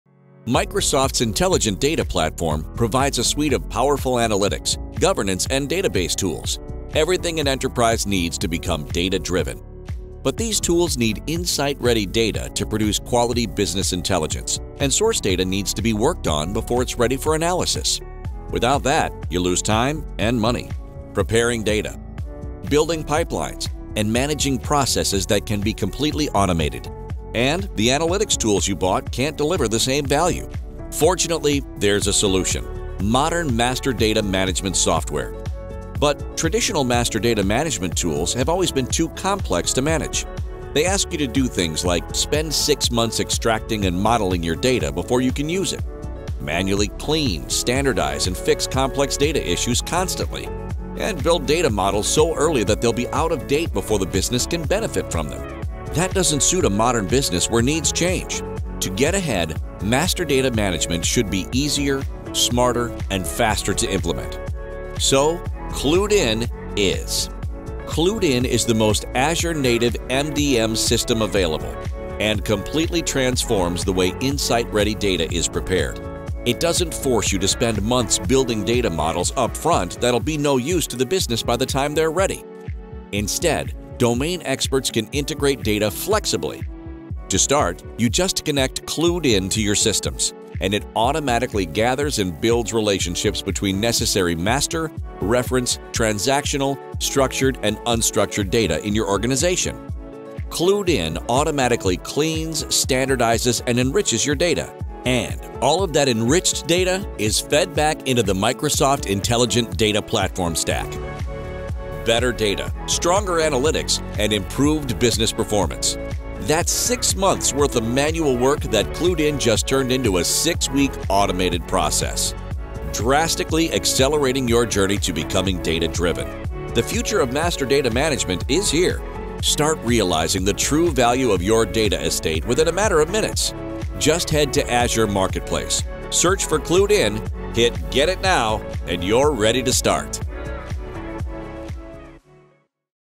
Microsoft Master Data Management and the Microsoft Intelligent Data Platform
Your data is the foundation upon which innovation is built and real-time decisions are made. Unlock the potential of your data with the Microsoft Intelligent Data Platform and CluedIn Microsoft master data management to get the insights you need, when you need them most.